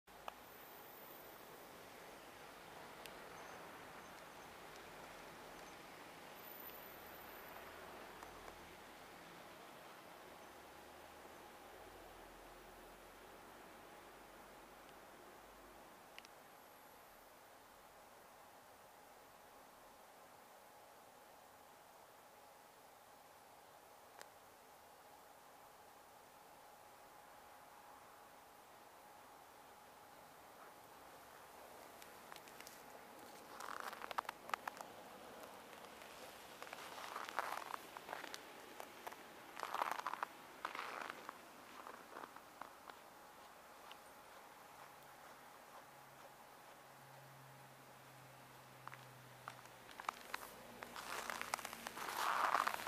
4D SÄÄ 22.11.2017 KEMIJÄRVI 3D VIDEO
-19 oli Saunavaarassa aamulla 09:30 aikoihin. Pilvistä ollut.